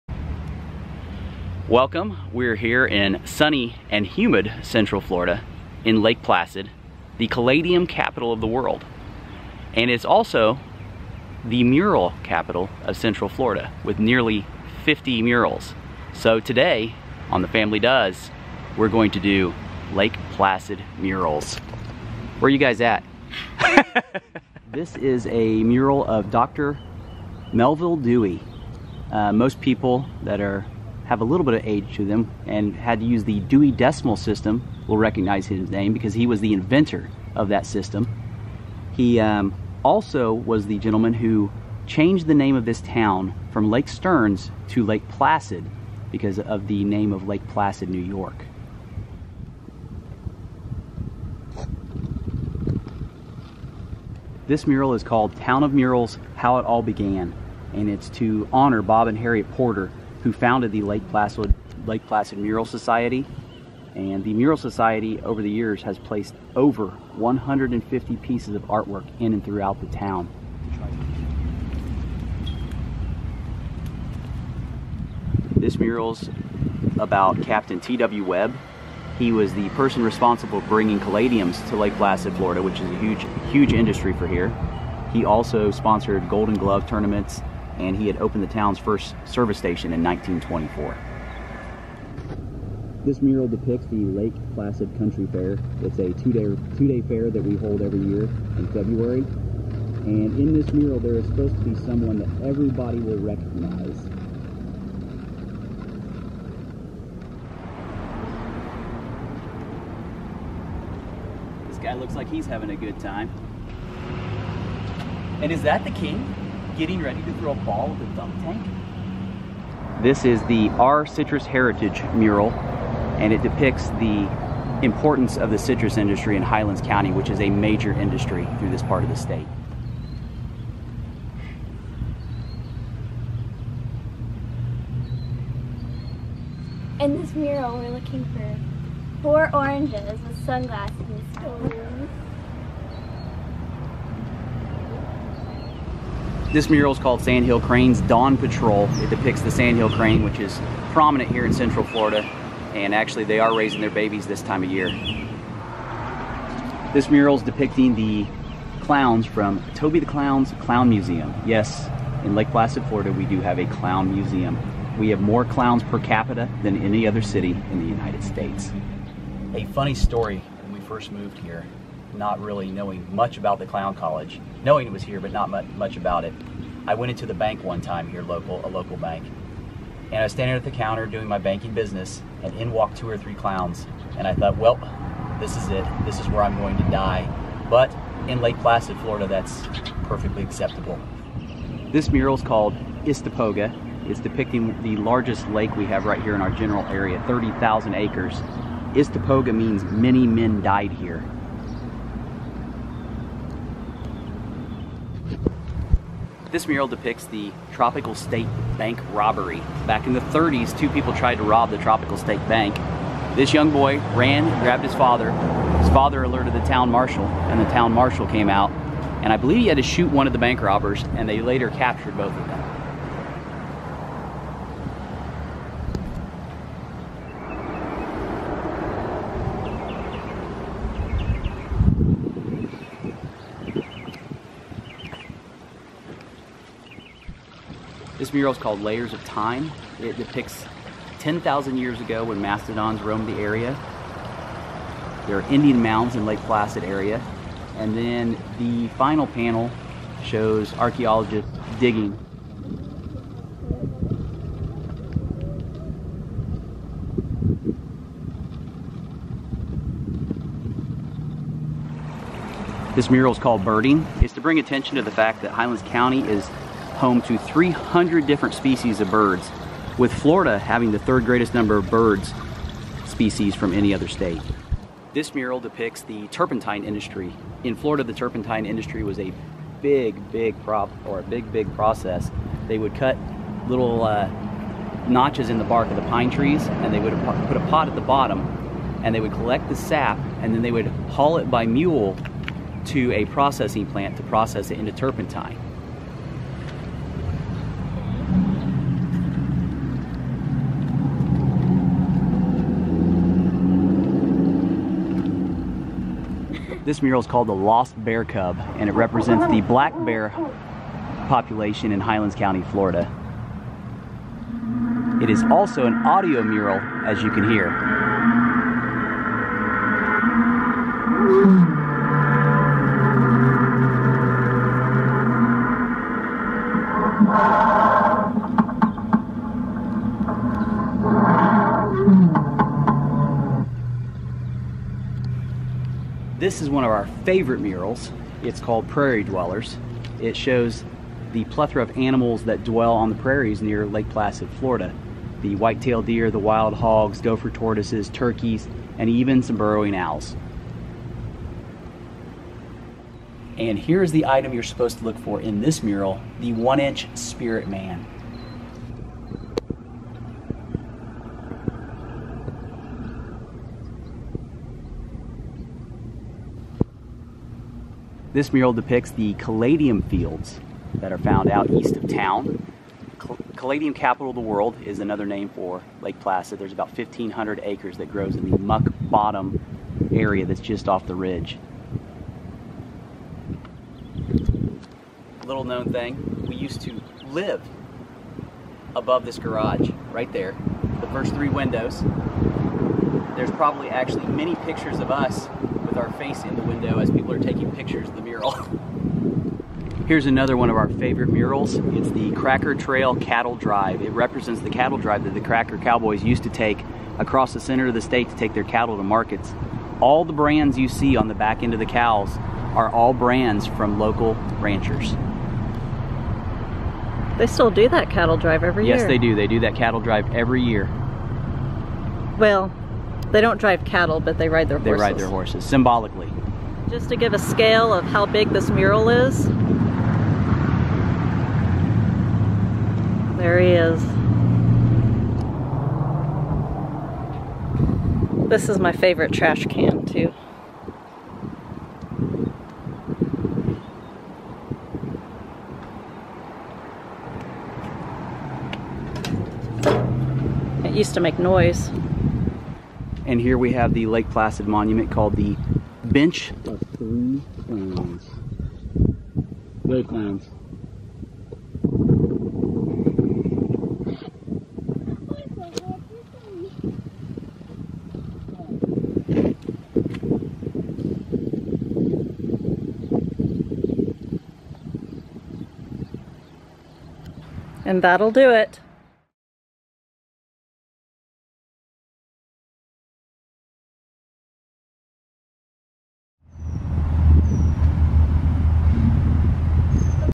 TheFamilyDoes Lake Placid Murals
TheFamilyDoes decided to explore Lake Placid Florida and it's fantastic murals.

That'll Do It.